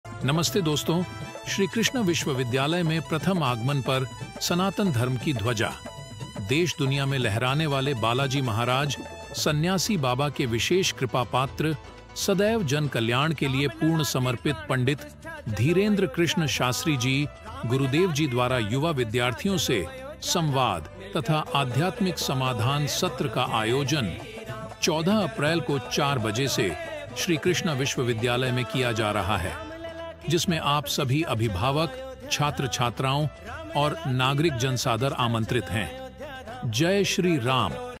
परम पूज्य श्री श्री 1008 श्री बागेश्वर धाम के पीठाधीश्वर के प्रथम आगमन पर 🙏🏻
परम पूज्य श्री श्री 1008 श्री बागेश्वर धाम के पीठाधीश्वर के प्रथम आगमन पर SKU 🙏🏻🙏🏻

हमारे श्री कृष्‍णा विश्‍वविद्यालय छतरपुर ,सागर रोड ( म.प्र.)में
दिनांक :- 14 अप्रैल 2024 🗓️
दिन :- रविवार को परम पूज्‍य श्री श्री 1008 श्री बागेश्‍वर धाम के  पीठाधीश्वर पं. श्री धीरेन्‍द्र कृष्‍ण शास्‍त्री जी द्वारा "आध्‍यात्मिक समाधान सत्र" एवं युवाओं के साथ प्रश्‍नोत्‍तरी में आप सभी सादर आमंत्रित हैं !! 🙌🏻❤️